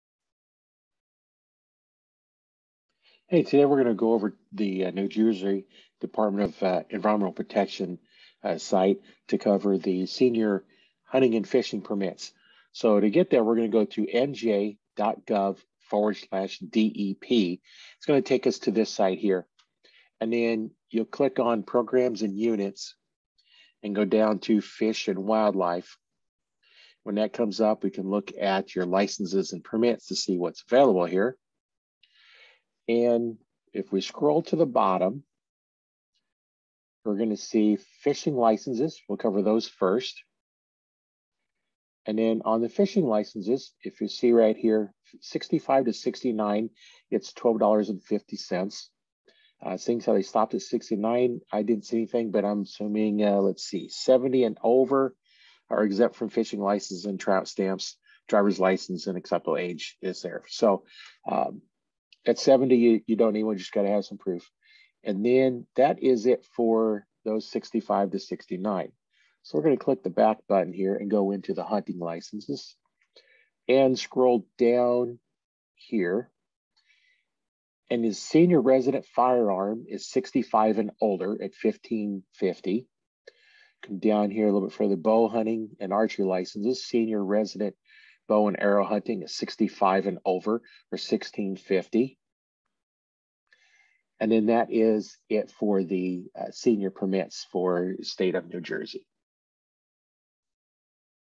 New Jersey Hunting and Fishing License
I show you who qualifies and how to get your New Hampshire Senior fishing license for $0.
For senior 70 years old you get a free fishing license.  and for seniors 65-69 they are $12.50
And for  the senior hunting for senior 65+ they have discounted licenses